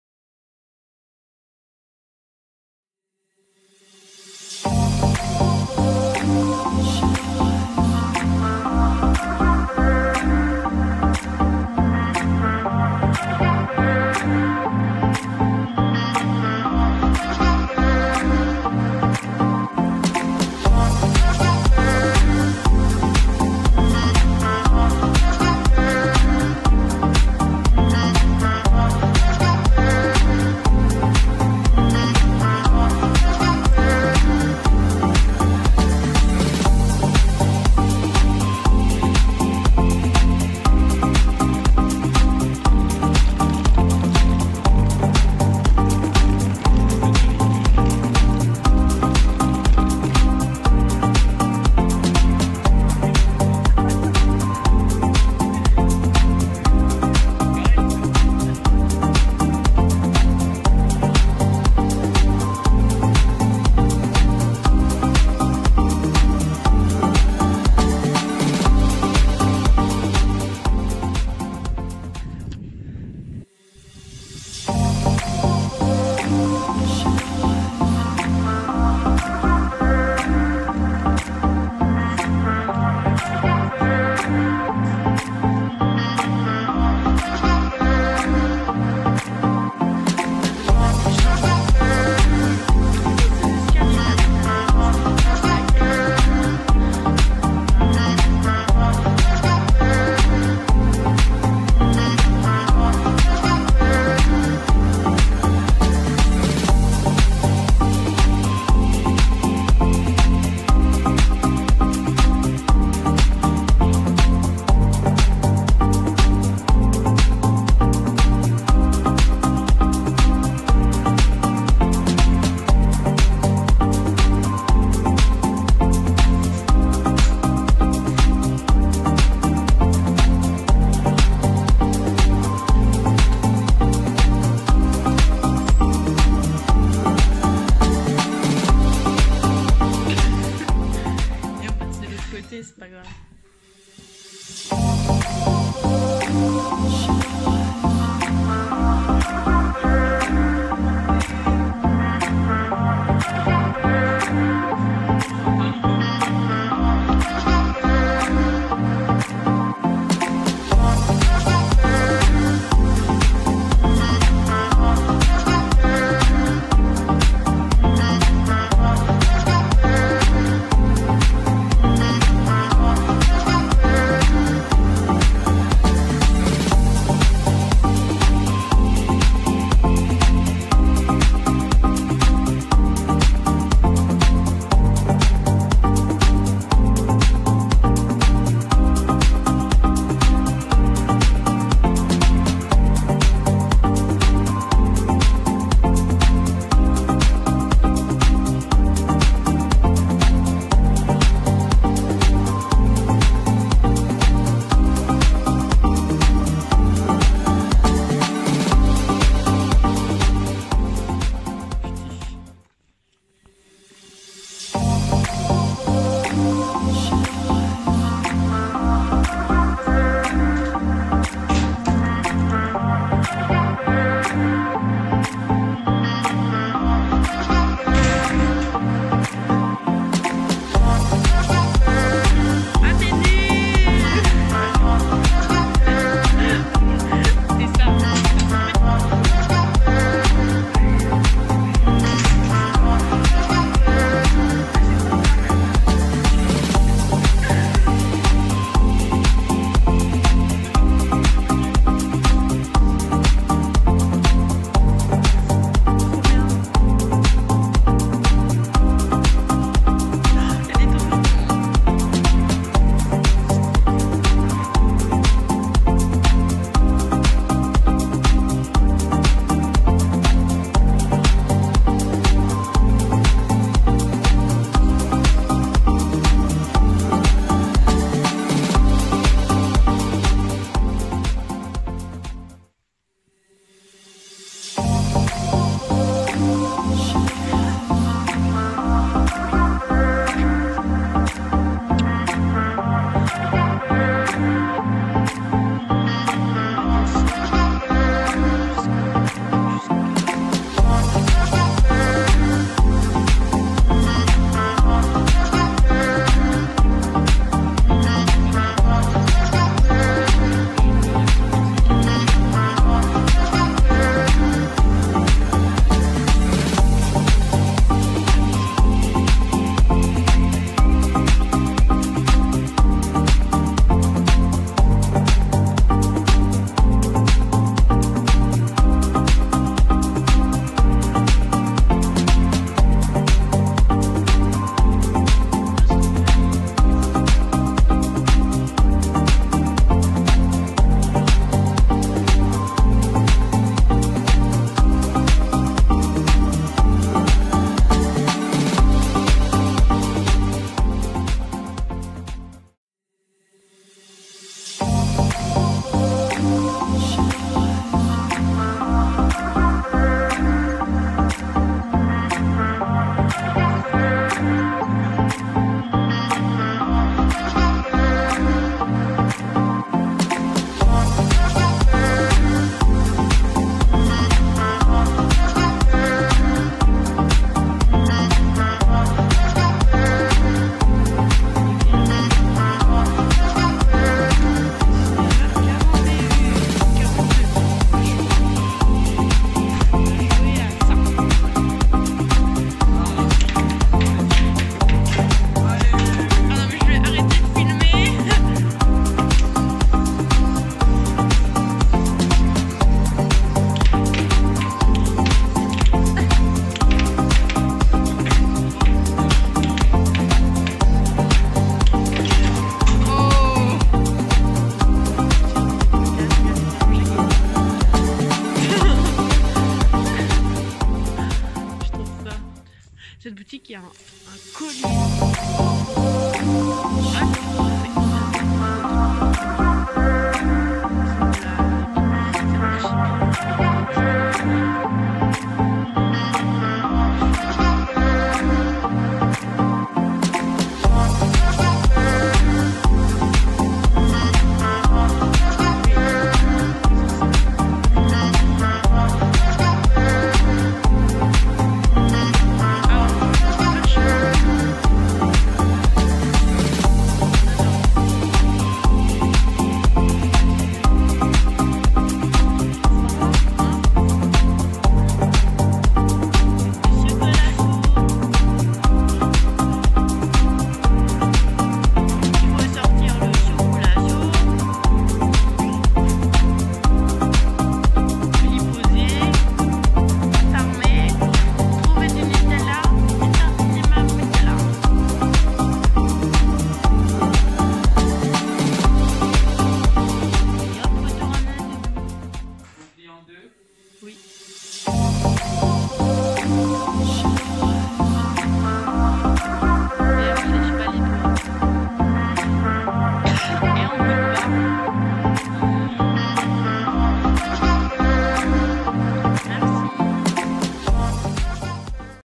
🔥Premier jour à valjoly🔥
Petit week-end à valjoly entre amoureux

Les Cottage de valjoly Eppe-Sauvage